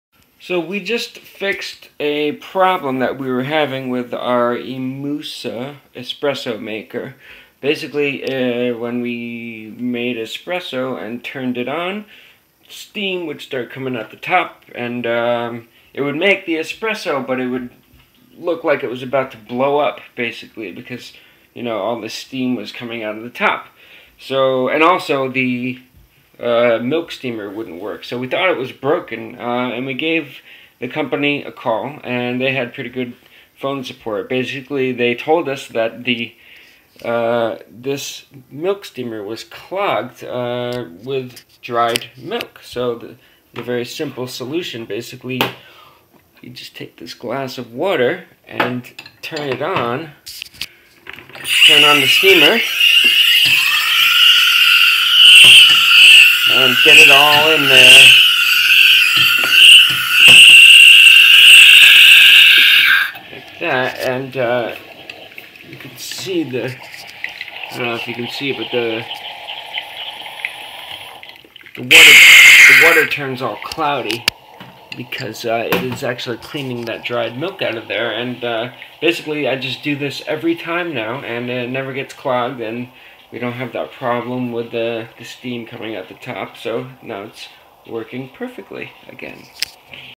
Imusa Espresso Maker problem FIXED! Overheating and steaming from the top!!
Our espresso maker started having this problem last week. It would get over-heated and steam would start shooting out of the top. It looked like it was about to explode! And the milk steamer also stopped making steam! But we figured out how to fix the problem. Basically we found out that over time, when the milk dries inside the steam tube, it builds up and eventually completely clogs the entire system. This is what causes the unit to start steaming and obviously the milk steamer stops working.

The solution is simple though, just take a glass of warm water and put it through the steam just like you would your milk. This will free up the dried milk instead the tube. Just do this every once in a while to prevent it from getting clogged up. This keeps the unit running smoothly! :)